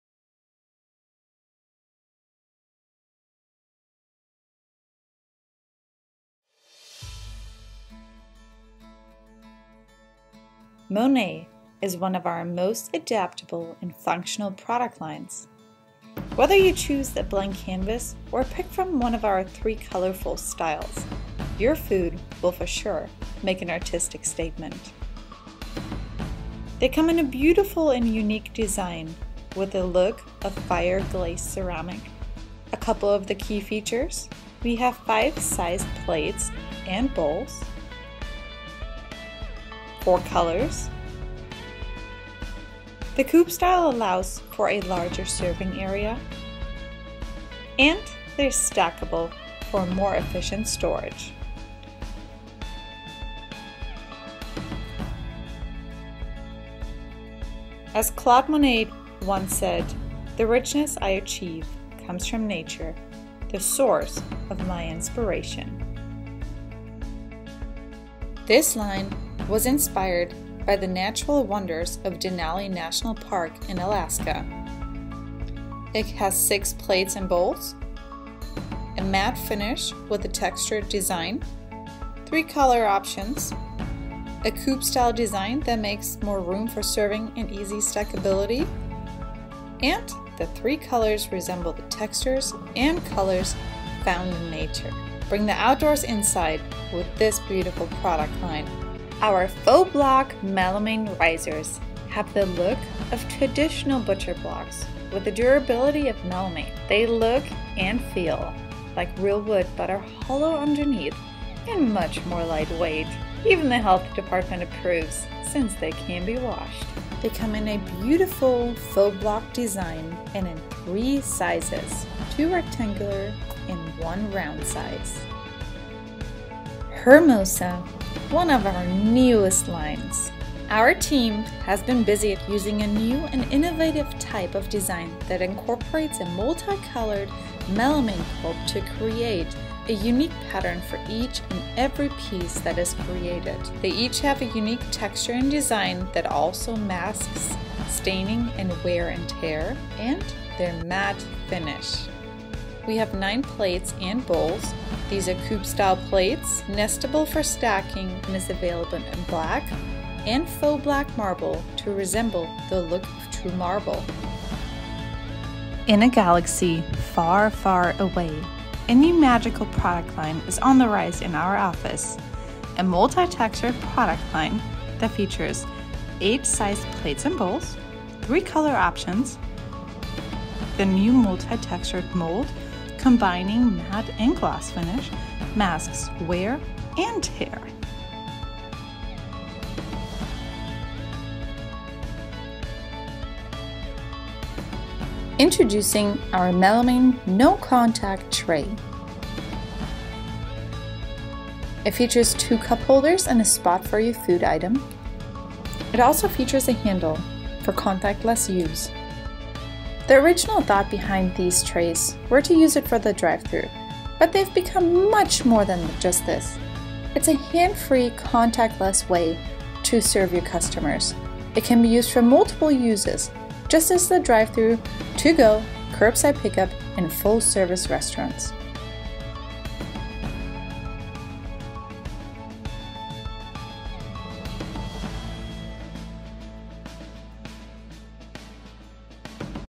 2021 New Products by Elite Global Solutions
Our team has been busy developing new products! Here is a short sneak peek to get a glimpse of our new products launching at the 2021 NAFEM Show.